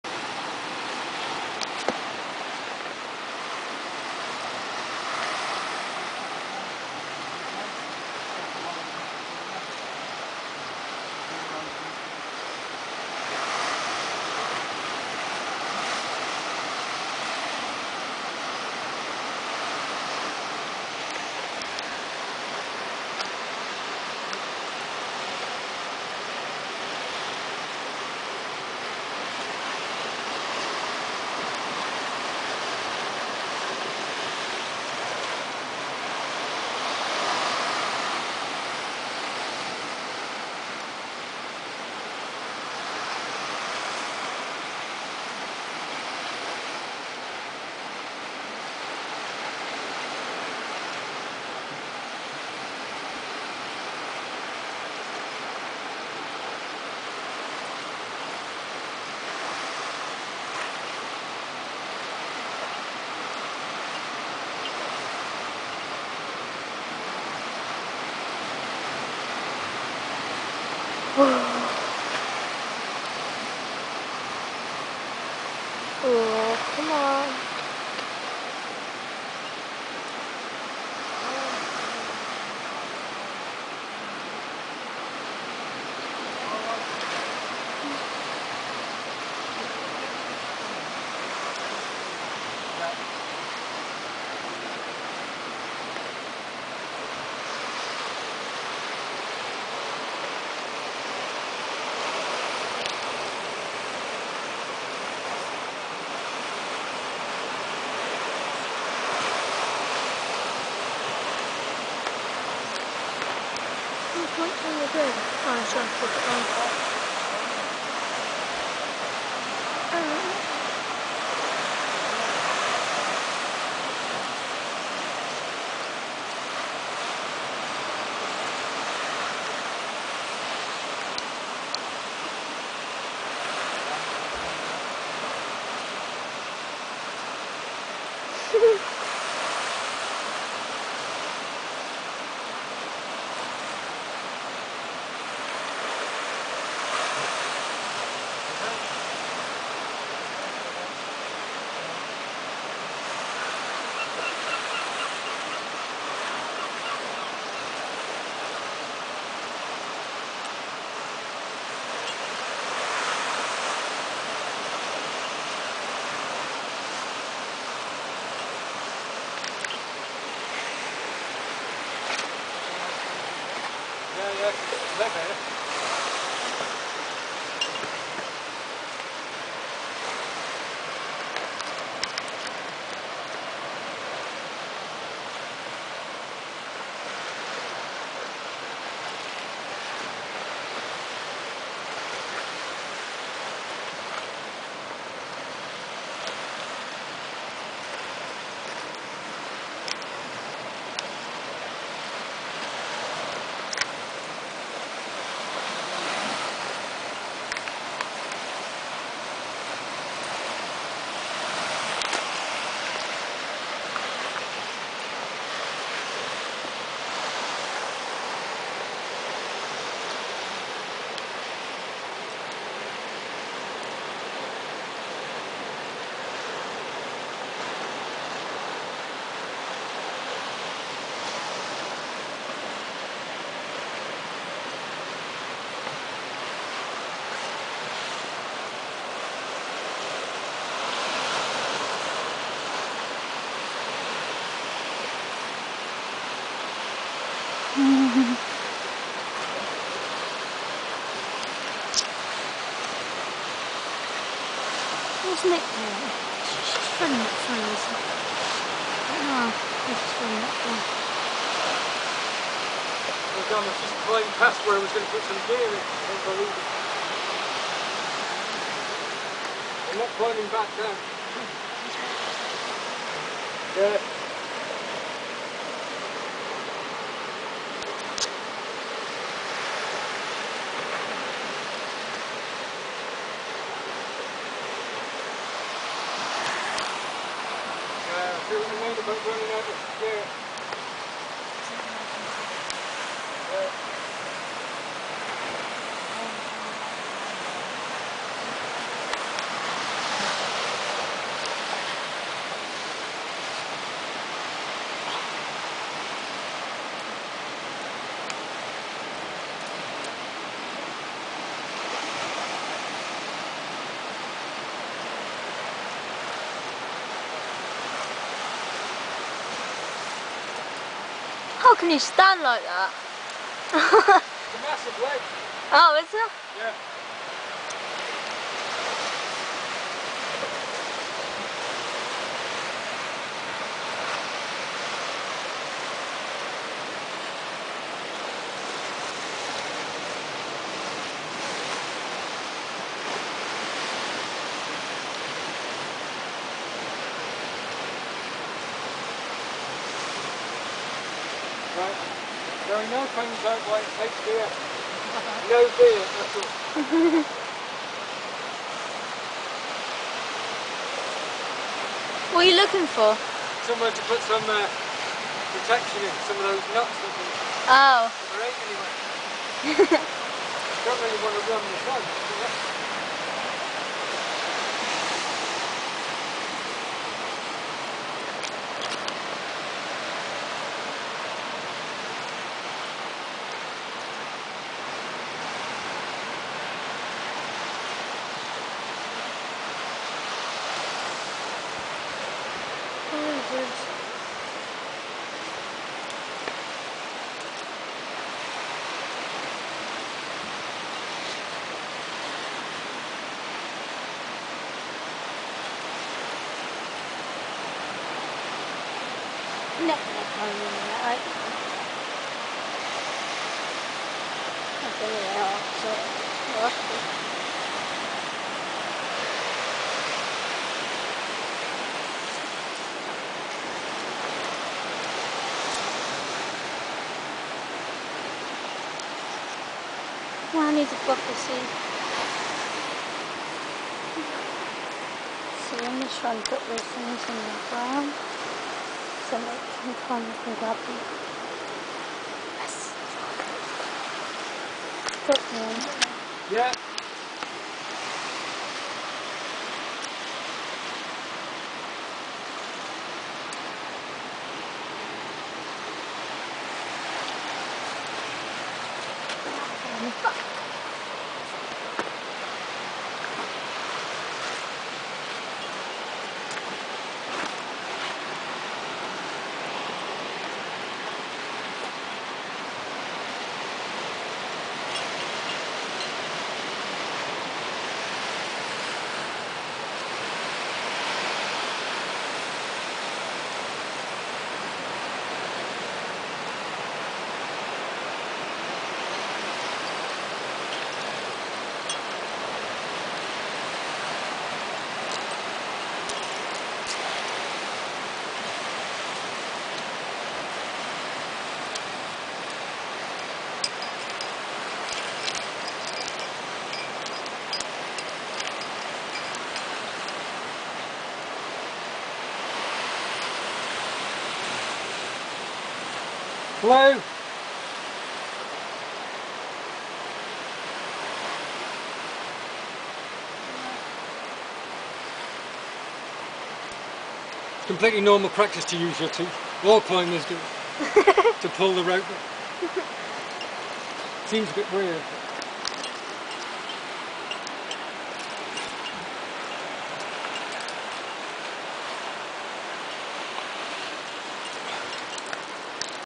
Mike Leading Haldrine Black Slab for First Time
Not much to clip your ropes to on second half, but a nice easy climb.